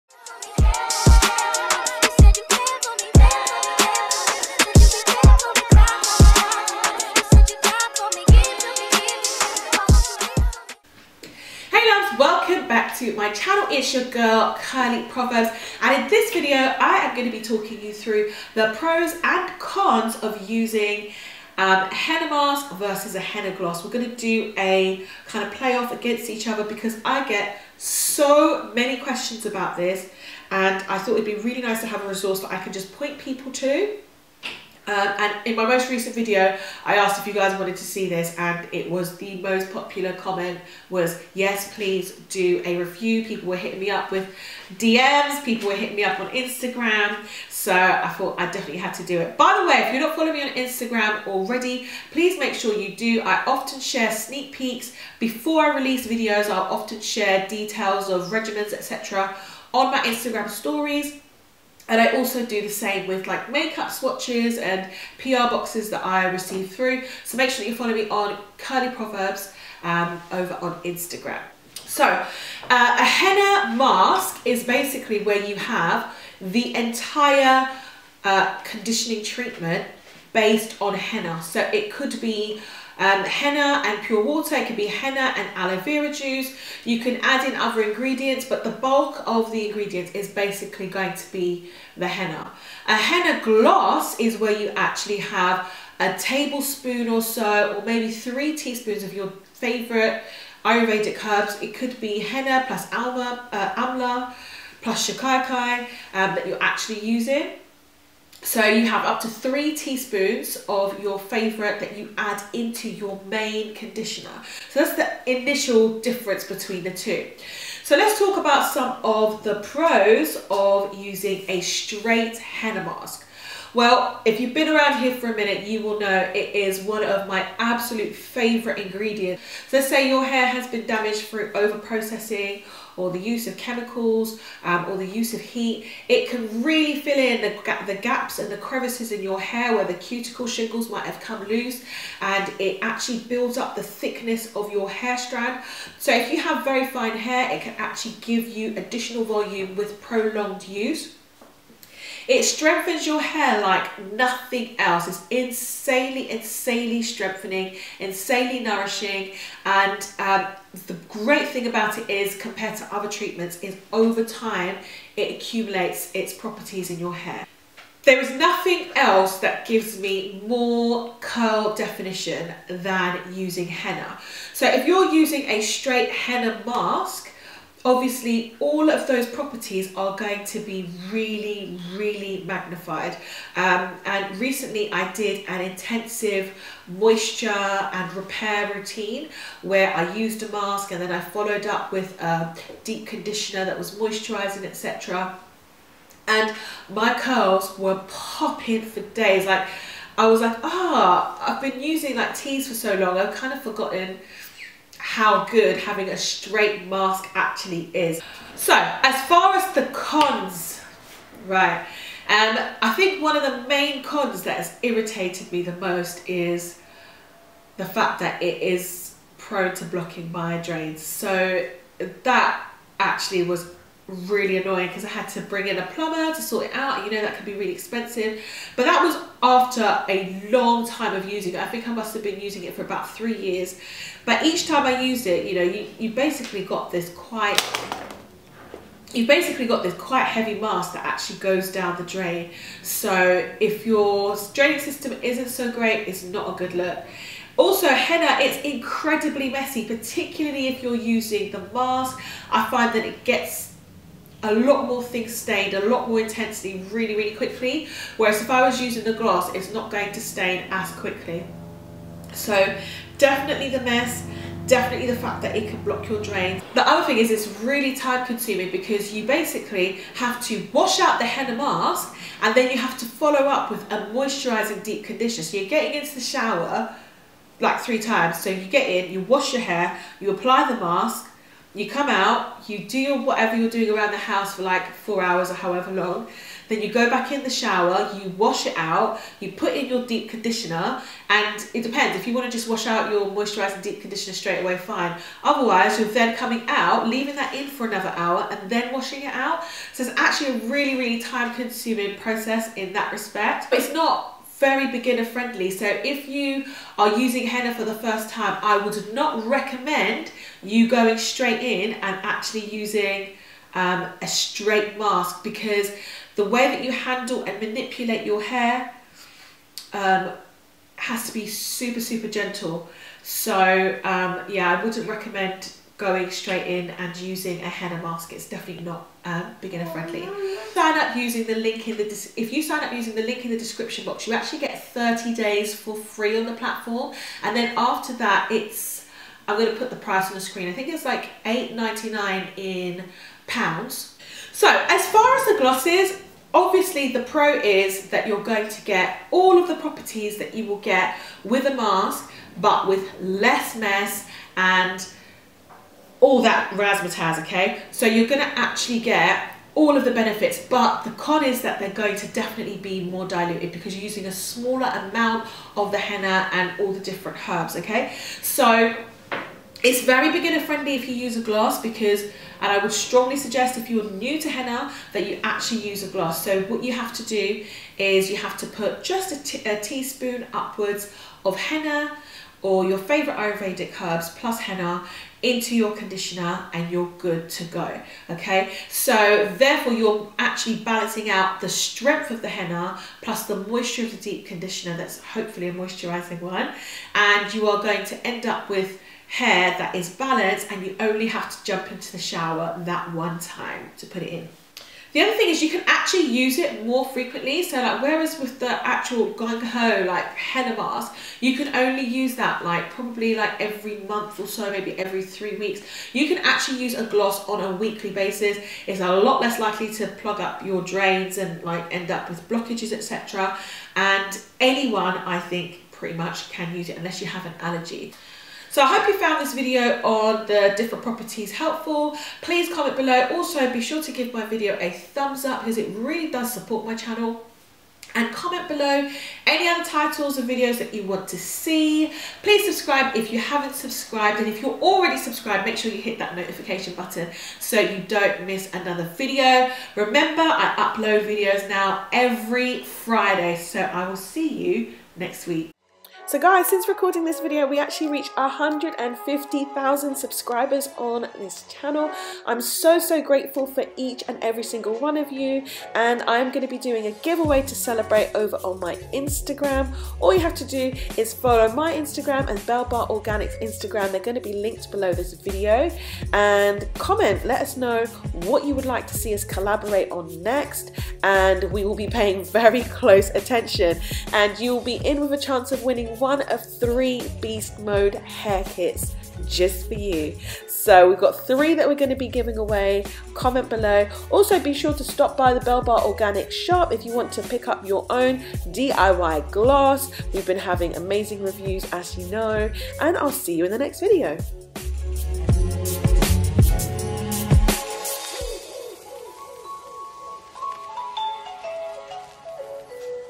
Henna Mask v Glosses - 150K BeastModeHair Kit GIVEAWAY
It goes well with a castor oil base or you could do the 4 ingredient oil and just add the onion and garlic essential oils. You could also add msm and fenugreek to this to make it a boss hair growth oil.

Amazon Shopping list with my must haves

In this video I go through the pros and cons on Henna Masks and Glosses.

MASK
PROS
Fills in Crevices of damage in hair
Thickens hair
Mad strength
Crazy definition

Cons
Blocked Drains
Messy
Time consuming
Not beginner friendly

GLOSS

Less messy
Beginner friendly
Les time consuming
Can be used more frequently
Less Likely to block your drains

Con
Henna / Ayurvedic properties are diluted